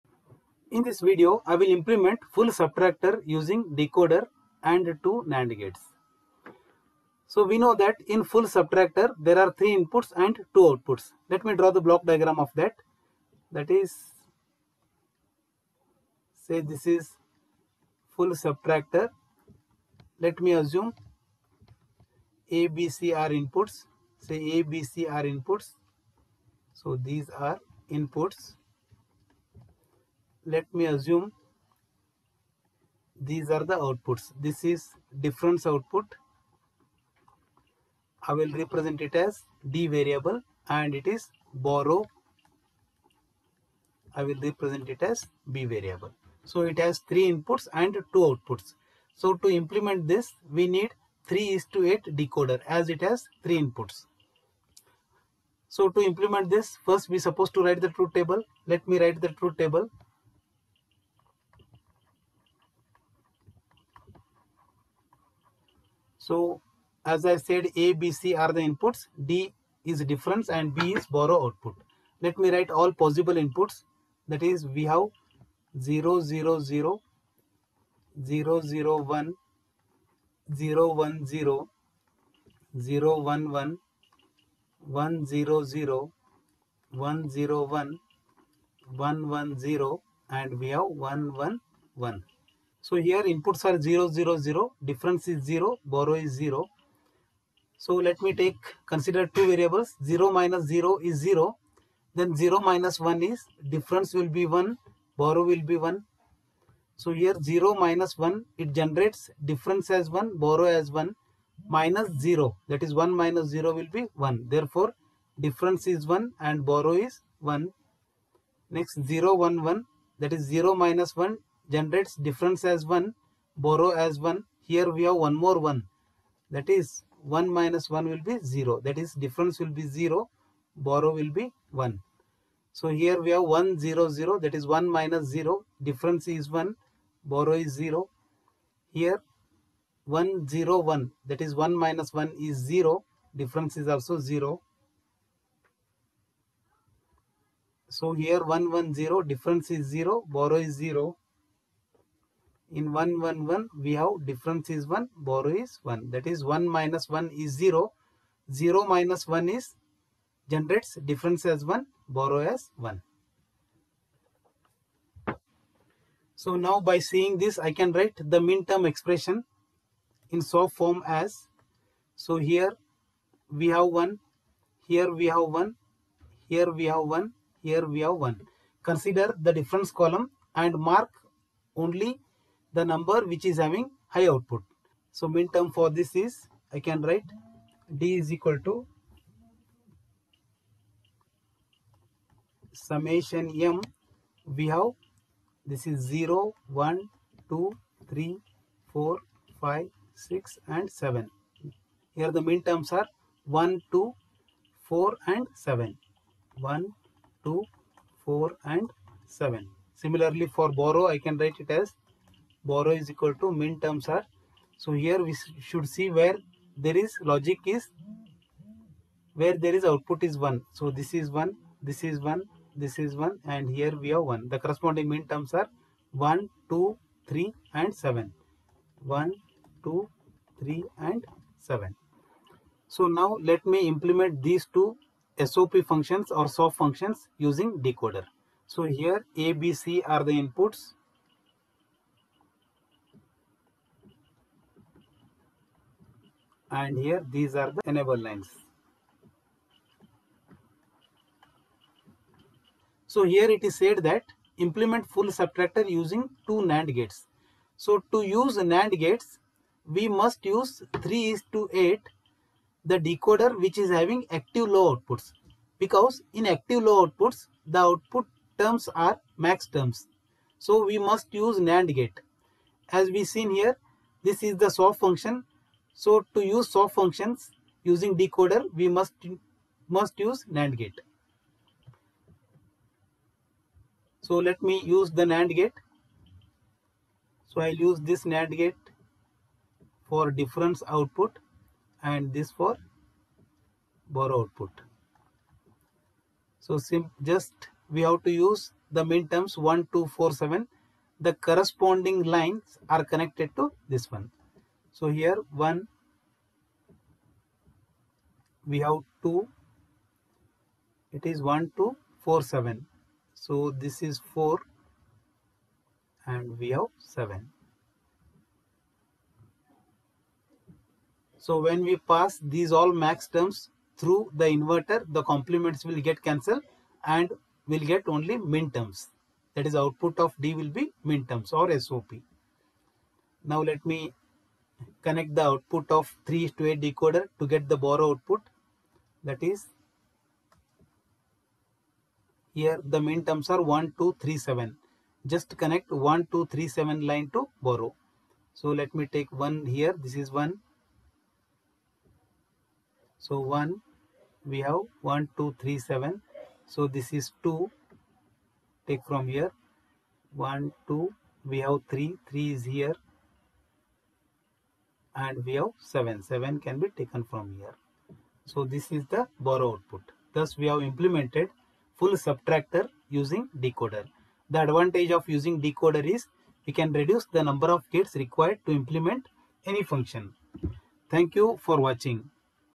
Full Subtractor using Decoder and NAND gates
Explanation,
Truth Table,
Implementation